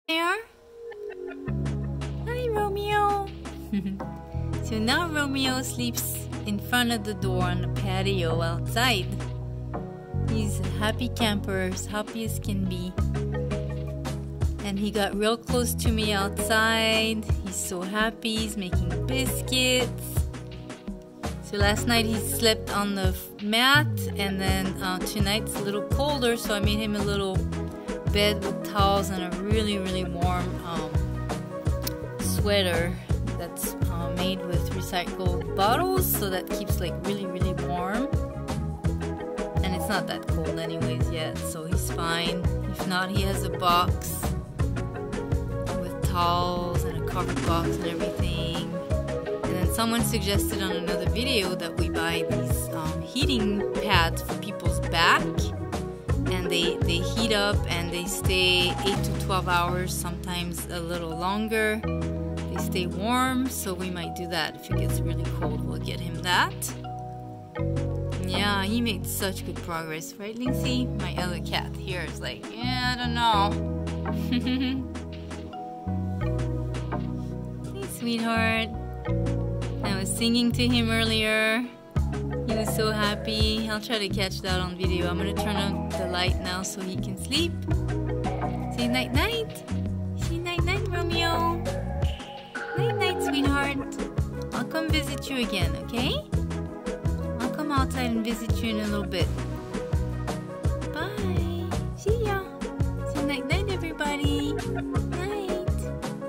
Snarf's (Romeo) bedtime story - Day 5 staywarm straykitty 🌃 🐈
Snarf's (Romeo) bedtime story - Day 5

It's getting cold in Central Florida! It was about 55 Fahrenheit degrees ... It's about to get in the 30-40s soon!! I will make sure this little sweety pie stays warm.
Remember to only use STRAW for OUTDOOR SHELTERS (no hay and no blanket no towels as they can freeze). It is not freezing here right now. I love how everyone has cared so much about him 💝

This is what a viewer was kind enough to write humanityhope
"The best heating pad things I've found are at Walgreens. They're the double size so it's 2 regular sized ones attached. They're the ones i was saying last 24 hours bc you fold them together & somehow the heat from each side keeps them hot longer. They come in a package of 3 for $7 & they're buy one get one half off so 2 boxes is a little over $10! They're where you find the regular heating pads at the store & are for backs!"